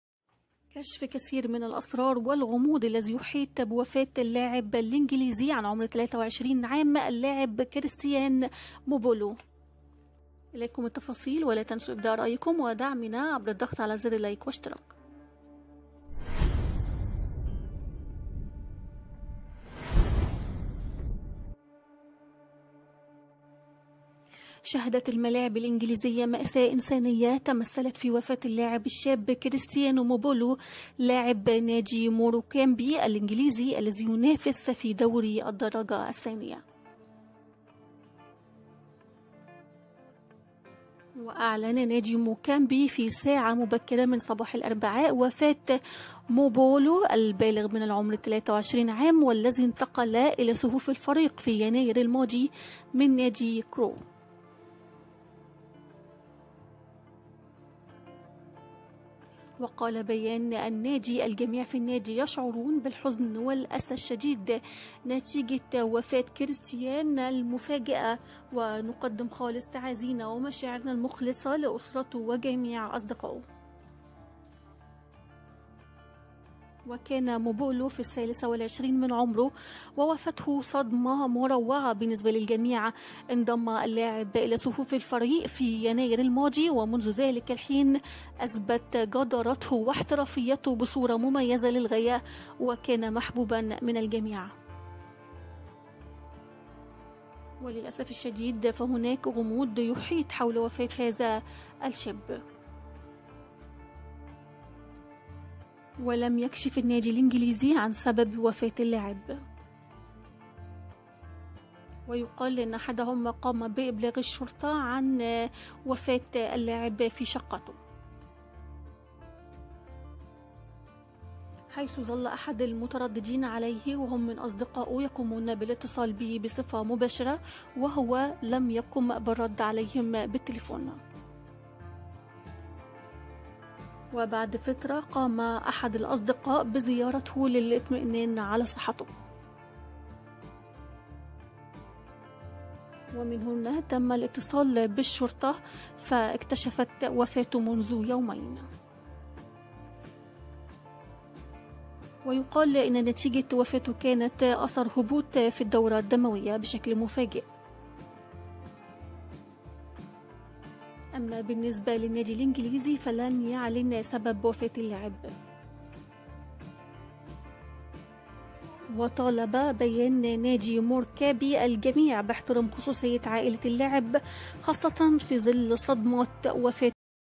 وفاة لاعب النادى الانجليزى عن عمر 23 عام
وفاة اللاعب كريستيانو مبولو فى ظروف غامضة عن عمر 23 عام  اليكم تفاصيل وفاة   لاعب النادى الانجليزى عن عمر 23 عام وسبب الوفاة الذى لم يعلن بعد